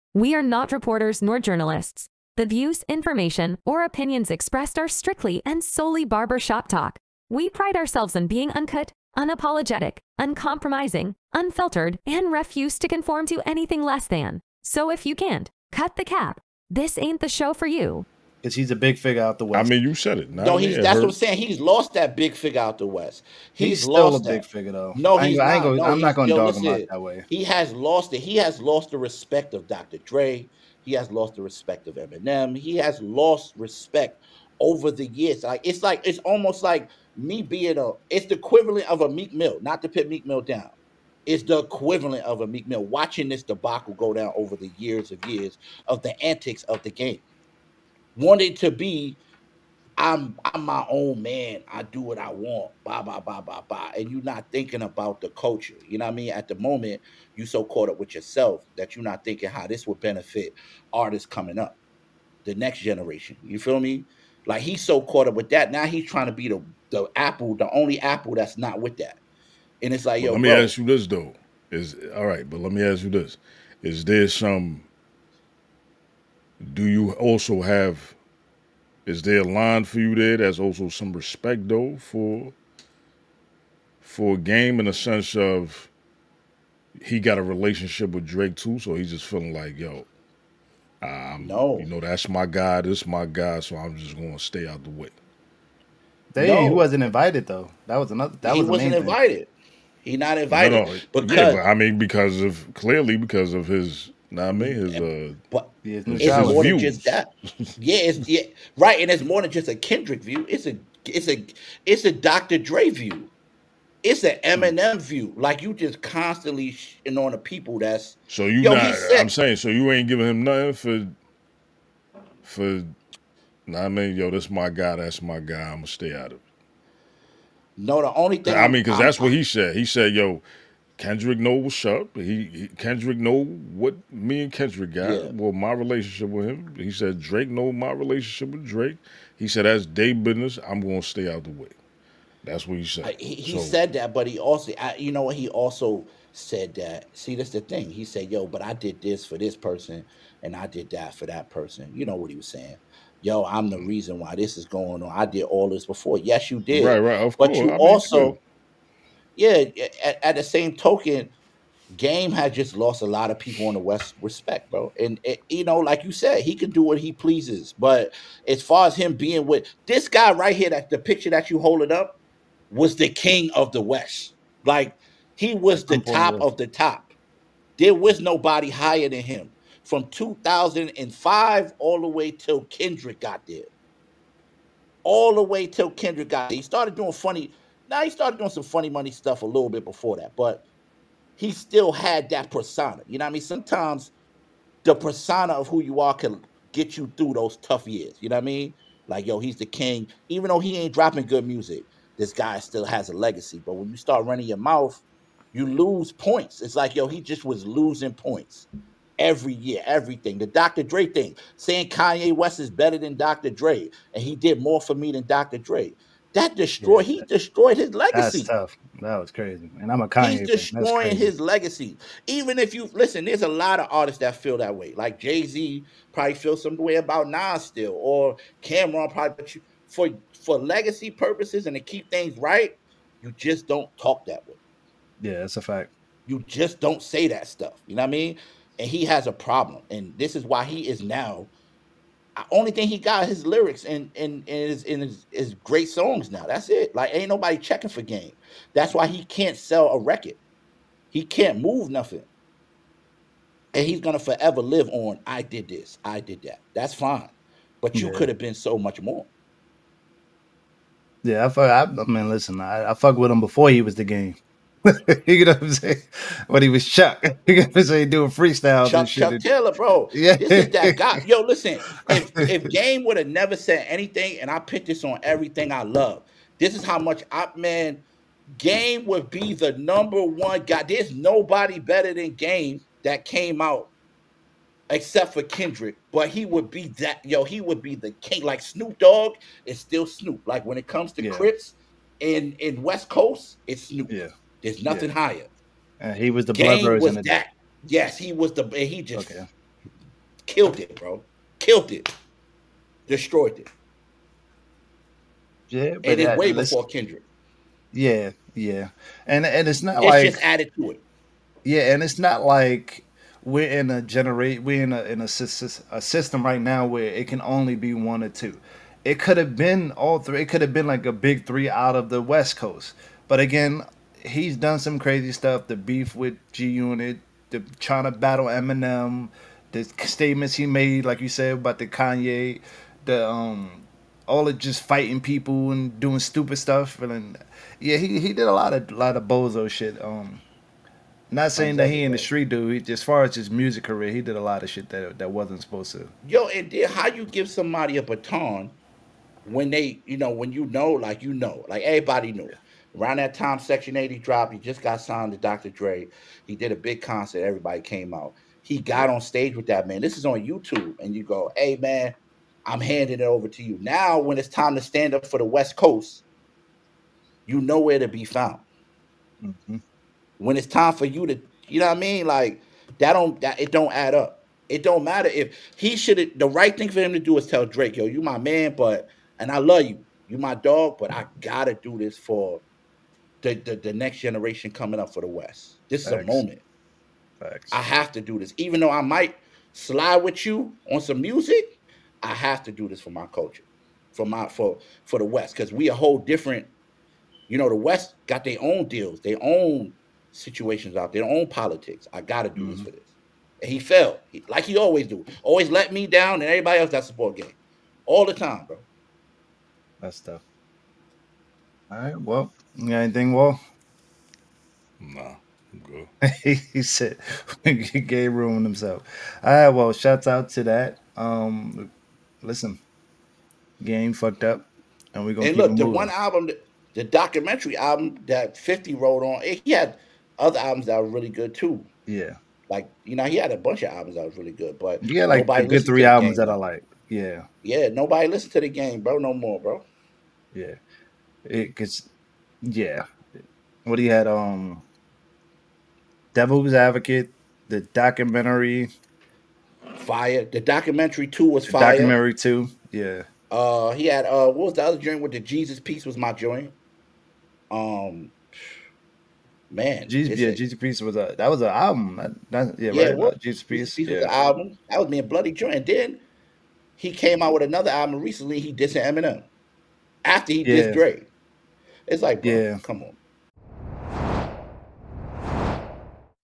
IS THE GAME NOT STANDING WITH THE WEST || CUT DA CAP || E144
This Week On "CUT DA CAP" We Discuss Current Topics, Alongside Our Very Own Segments Cut Da Cap Feature Spotlight, Cut Da Reacts, Cut Da Track & Cap Or No Cap Segments! New Episodes Streaming Live Every Wednesday 9:30pm.

FOLLOW - @Cut_Da_Cap @IamSTATUZZ @Bashworld @StrategicFilmz @WALxGoogle5ive